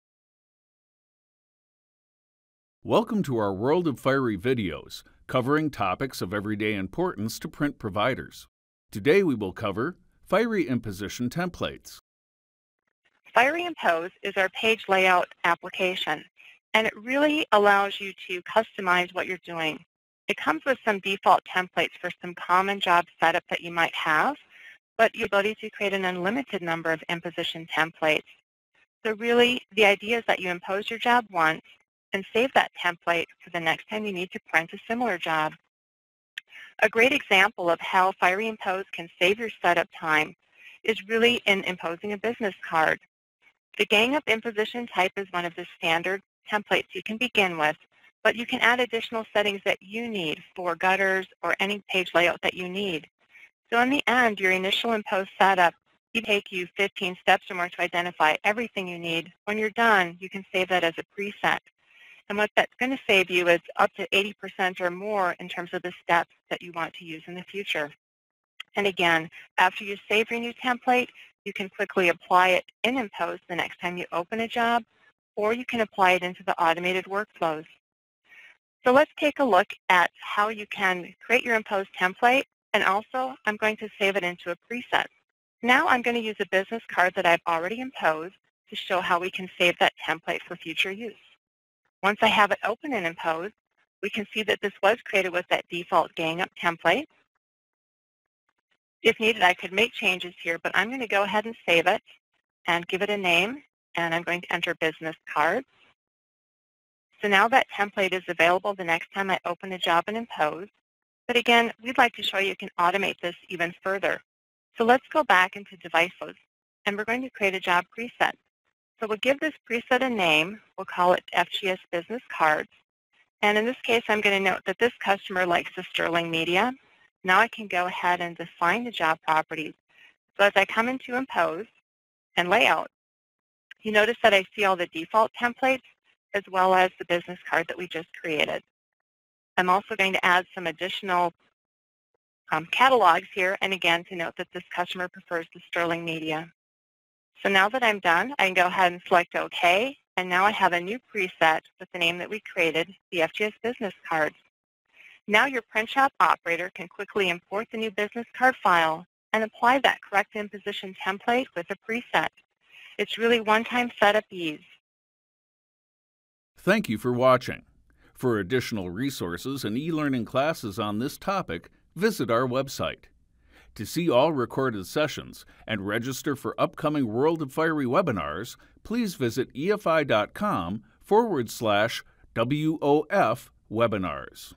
World of Fiery shorts – Fiery Impose templates for business cards
Lay out gangup jobs straight from Fiery Command WorkStation using customized imposition templates. Learn how you can reduce your workflow steps by 80% with Fiery Impose.